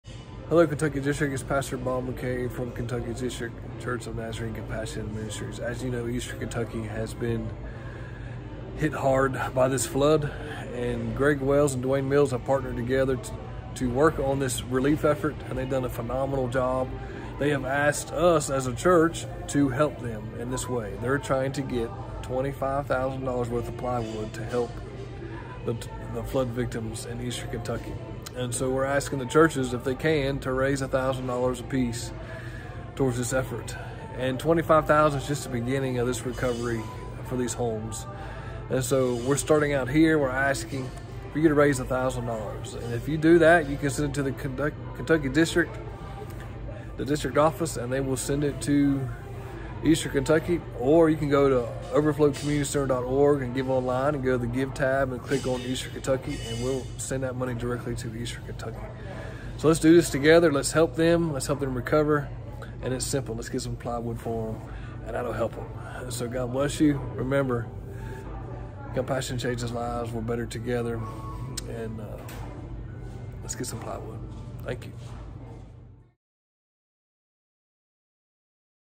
Plywood for Eastern KY
We are helping raise funds to purchase plywood to help families effected by floods of Eastern KY work on their homes.